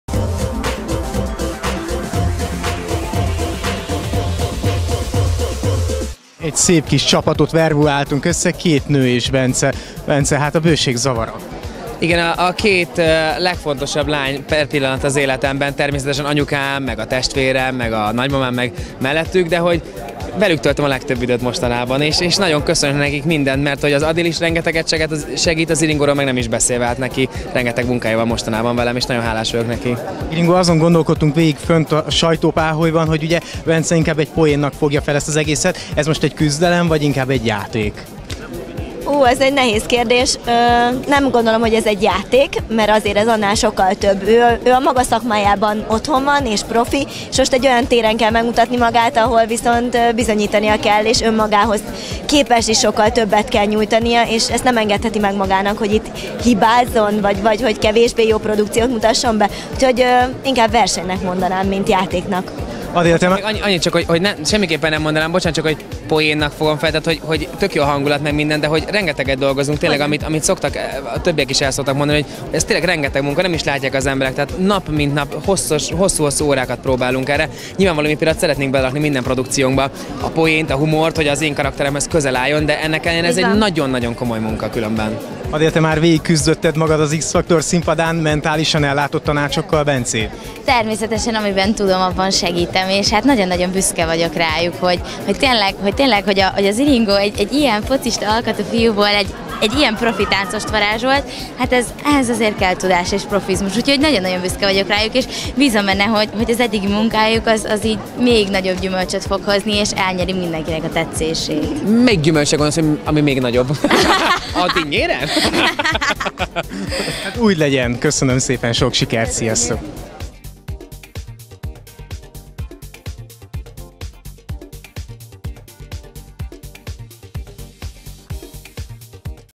MosaicOnline - Südi, Istenes és Csobot Adél mentális tanácsai
A Szombat Esti Láz első adása után kaptuk el egy interjúra Südi Iringót és Istenes Bencét, akinek  párja, Csobot Adél is kameránk elé állt. Míg Iringó elmondta, hogy verseny vagy játék-e a műsor, Bence jól összezavarodott abban, hogy ki is a fontos nő életében, Adél pedig mentális tanácsairól mesélt.